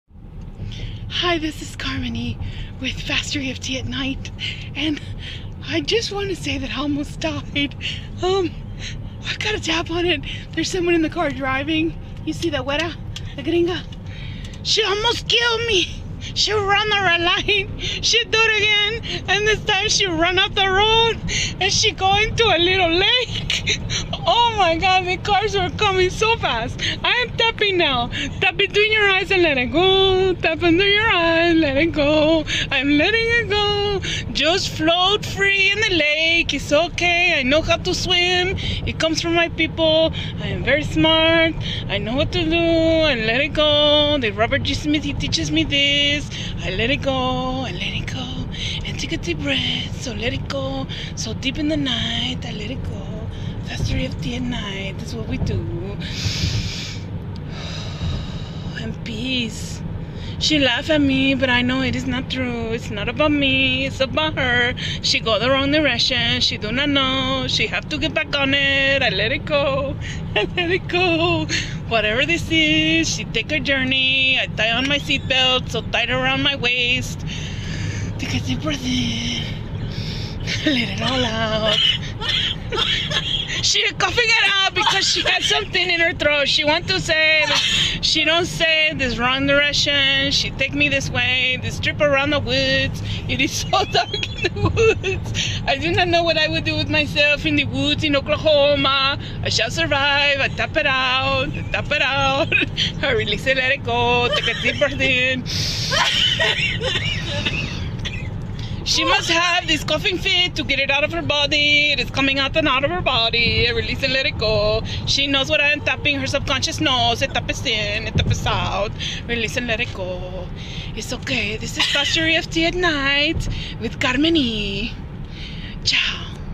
FasterEFT After Dark : Red Means No Green Means Go : Part 1
When we are quiet our thoughts run free, if thoughts are causing havoc, sleeplessness, discomfort, stress, or perceived pain, Tap Into The Night with Faster EFT. Let's keep it interesting - while stories are great, the idea of recording yourself in the middle of night, in the middle of your trance can be so much more fun. All goes, share your wake up videos here. The idea is to keep the light low or pitch black.

I'll start it off by saying 2 years ago, I would have never been joking around about this - today - THANKS to Robert G. Smith and FasterEFT I'm OVER THAT SHIT. And this my friends is CARMENdy (as in comedy, but my name at the beginning - ok whatever, I CRACKMYSELFUP). In the midst of recording these silly antics and stress related incidents, I found my CONNECT-THE-FASTEREFT-DOTS - and thus the 'Ripple Effect' began.

So if you know my HT story, the words I'll use below all relate to how I triggered myself in the midst of almost plunging to my 'death' in the backwoods of Okley-HOME-ahhhh with 'Beth' at the wheel.

It was a dark night, the tank was running on close to empty - Elizabeth Thompson abducted, snatched - cohersed me into her vehicle to go for a hay ride. RED MEANS NO - I TOLD HER - GREEN MEANS GO - PLEASE GREEN GO - RED NO! The vehicle was running on spit fumes as 'Beth' prayed her way to the gas station - and I tapped 'I believe it's right around that corner'. There it was around the bend...ANY gas station. We gassed up and were safe to make our way back to the hotel - low and behold 'Beth' decided I needed to see the scenic route, through the fields and in the lake. Hence the first video - pitch black. Video Part 1 - Oh, and ask her about the white pants next time you see her - jee jee.